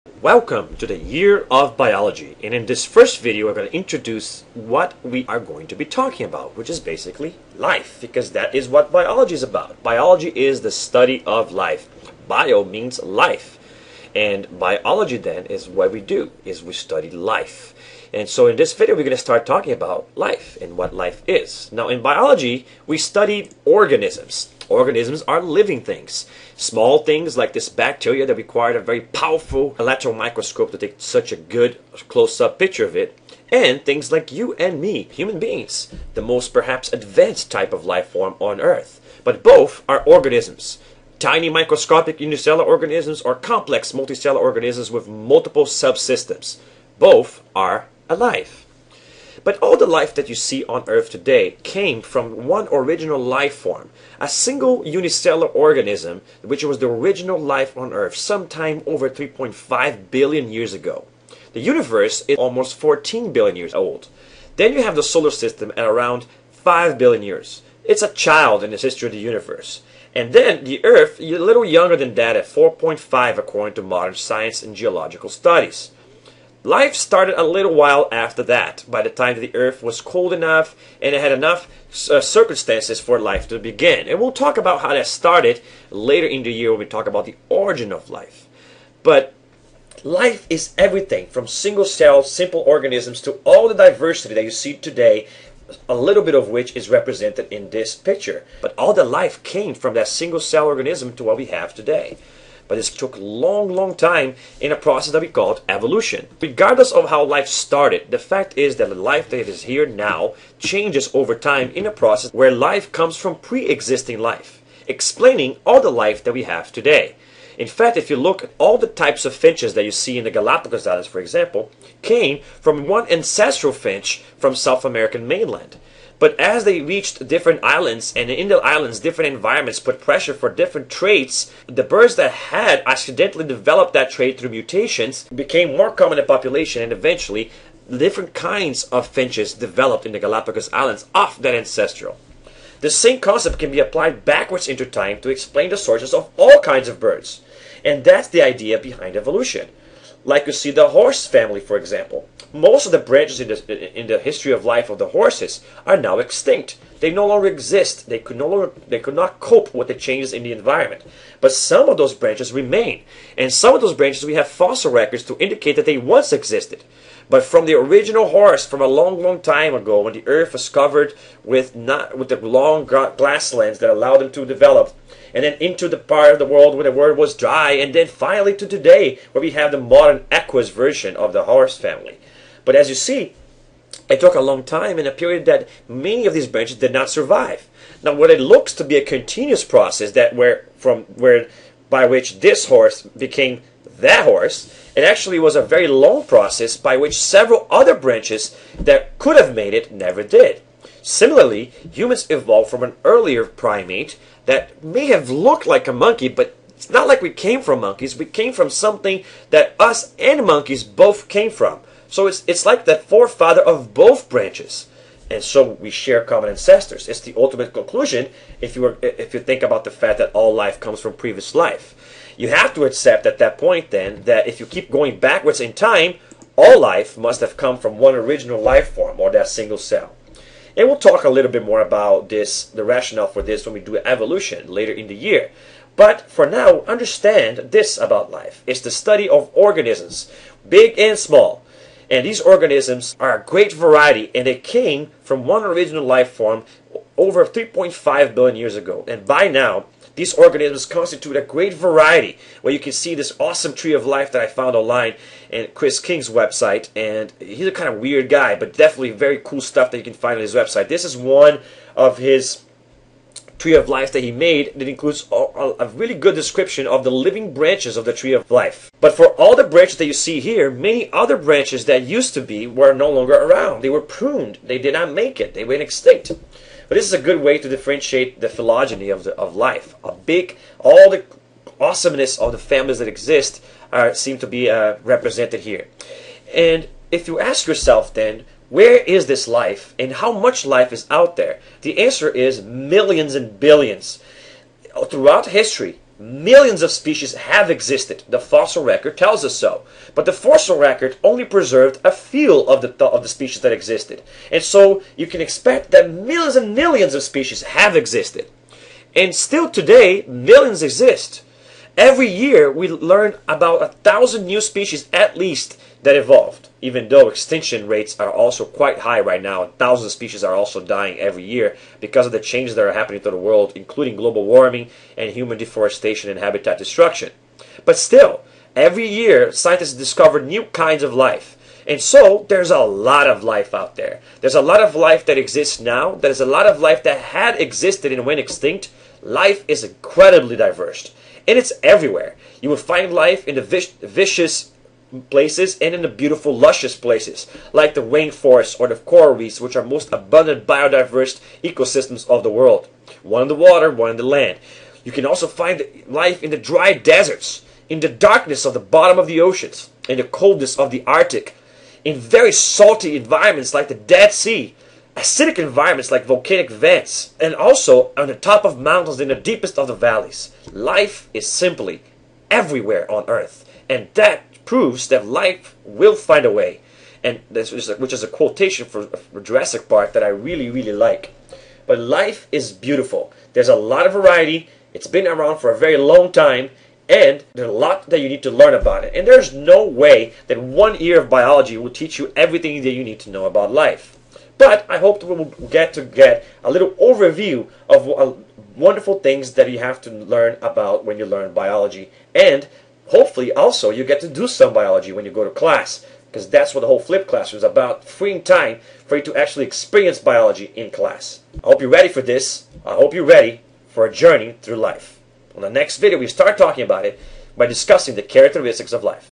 Introdution to Biology
This video is part of "The Nature of Life" Biology lecture series. To see the full list of videos,

Summary: Mr. Lima introduces the first biology lecture series by talking about life.